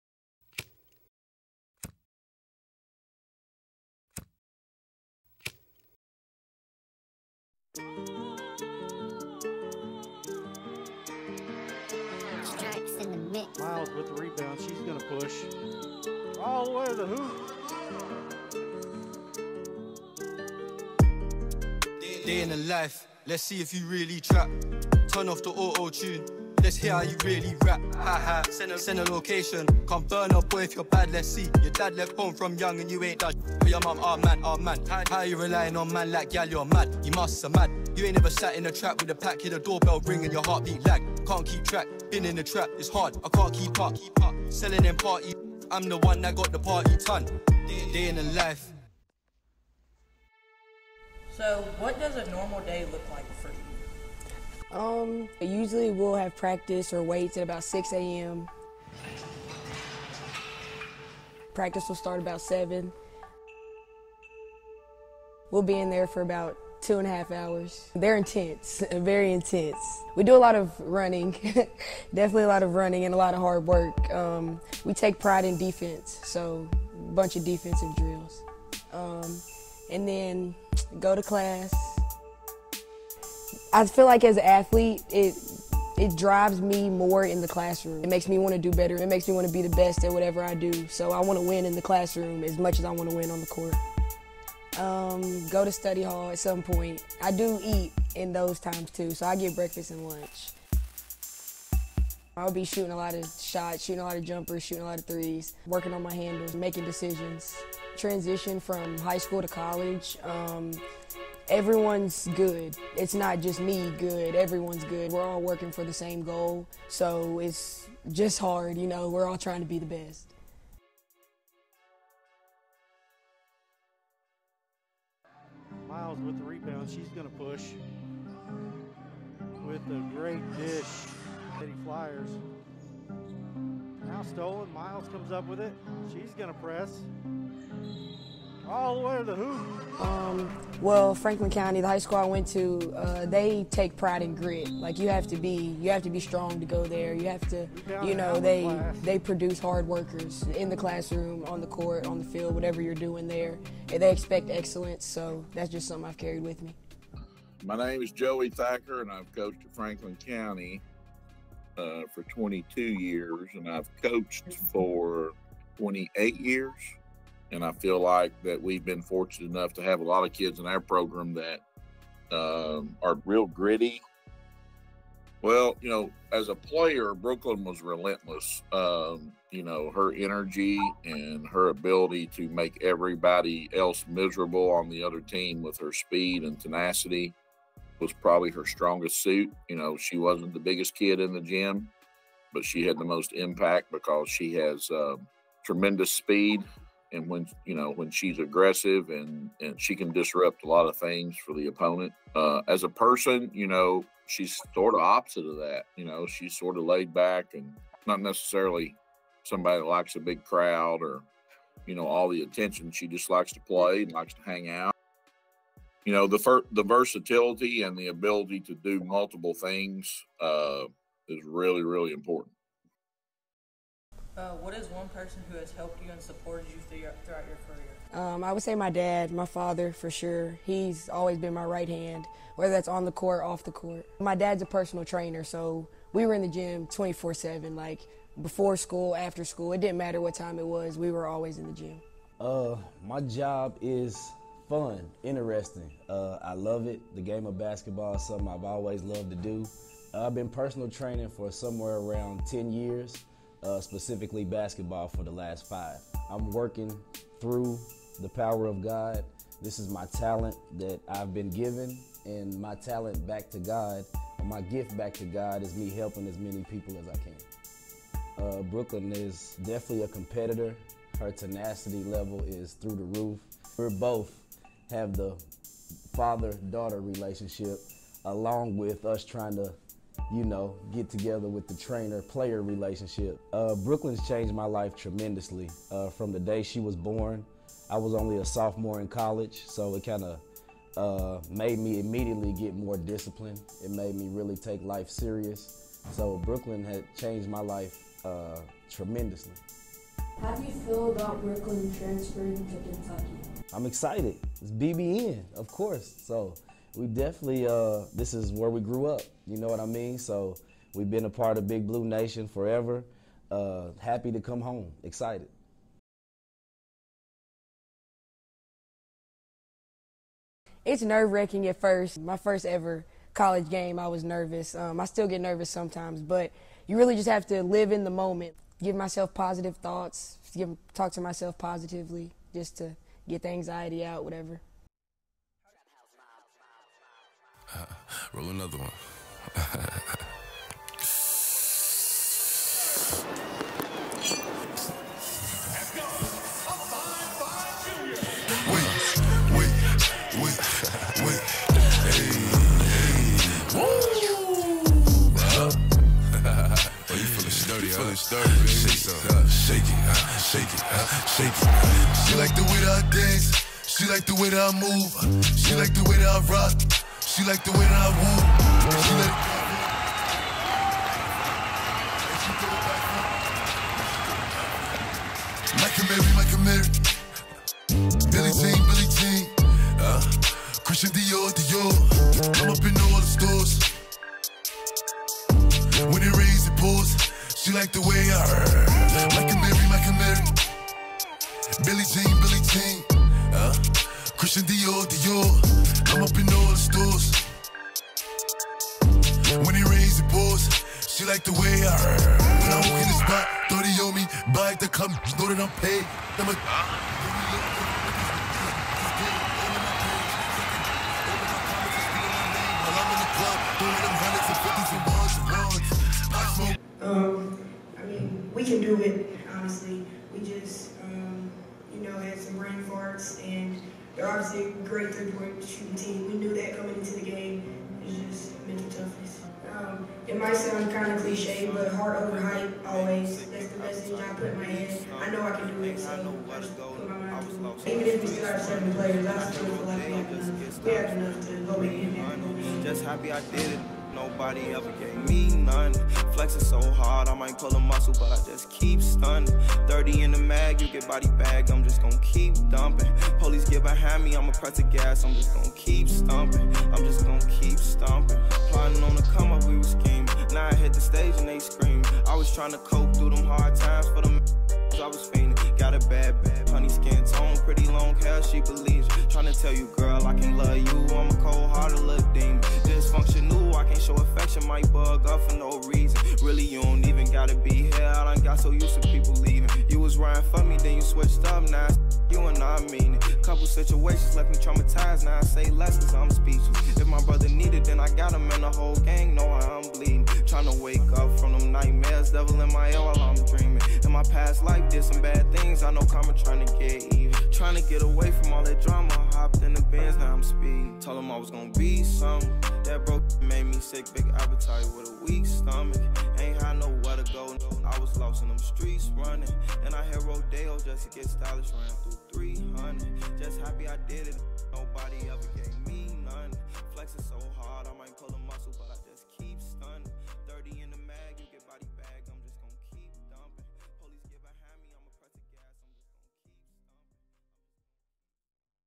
DAY IN THE LIFE - A documentary on Brooklynn Miles
This compelling documentary invites viewers to experience a glimpse into Brooklyn Miles' life, revealing the indomitable spirit that propels her forward in the pursuit of excellence. "Day in the Life" is a celebration of passion, perseverance, and the indelible mark that one remarkable athlete is leaving on the storied legacy of women's basketball at the University of Kentucky.